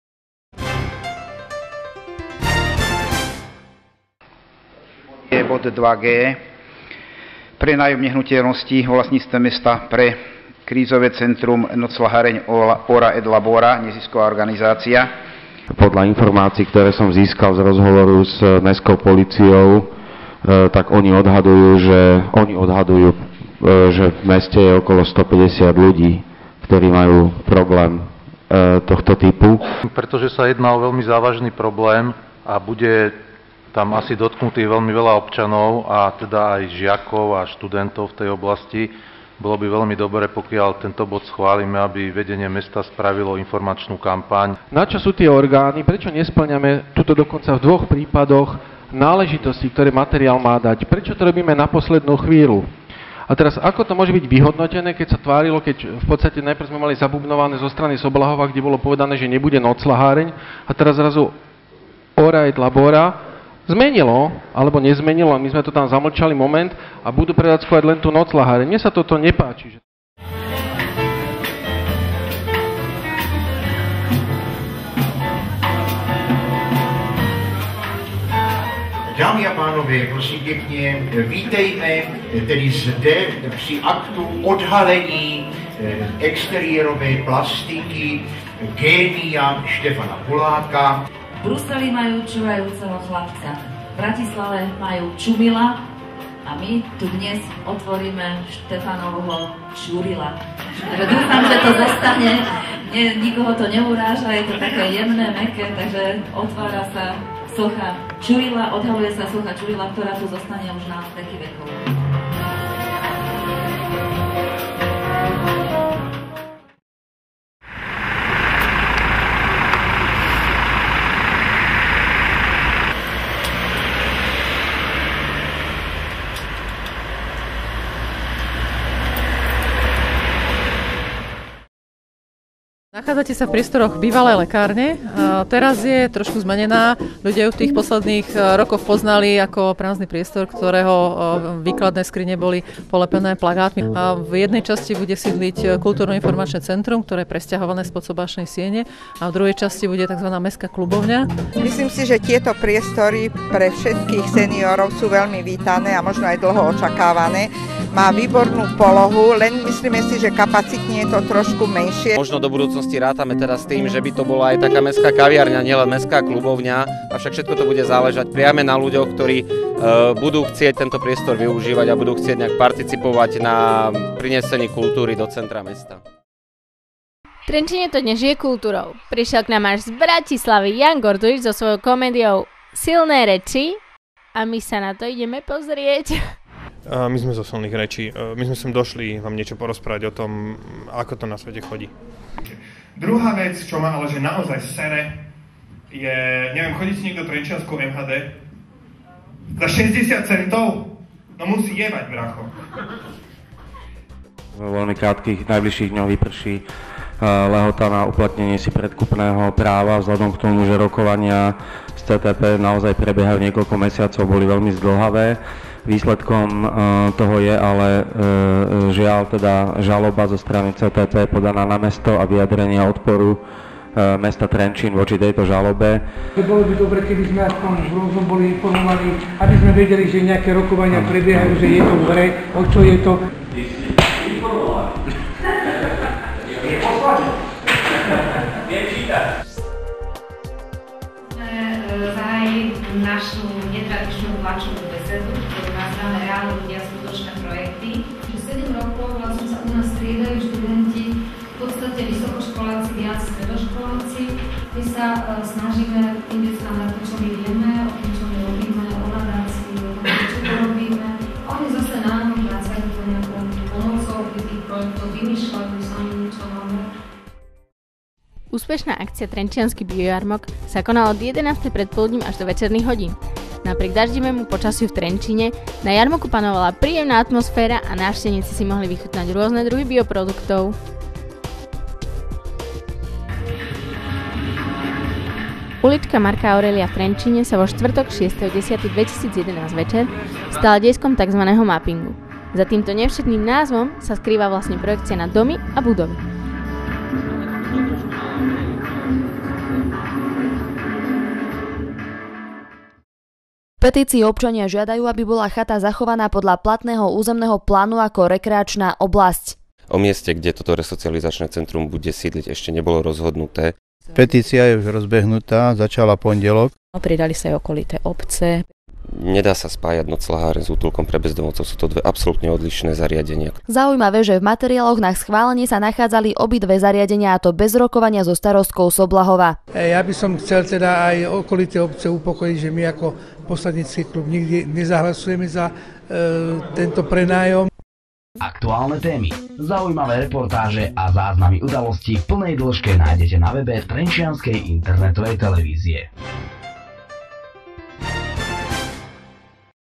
Aktuálne reportáže 26.10.2011
Prehľad aktuálnych príspevkov:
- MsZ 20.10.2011
- Trenčiansky Čuril - odhalenie sochy
-Obrovské divé prasa zrazené pri Laugaríciu!
- Otvorenie Mestskej klubovne v nových priestoroch KIC
- Silné reči v Trenčíne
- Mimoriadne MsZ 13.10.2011
- Reálni ľudia, skutočné projekty - tlačová beseda Trenčianskej nadácie
- Trenčiansky BIO jarmok
- "Mapping" v uličke Marka Aurélia
- Chata pre bezdomovcov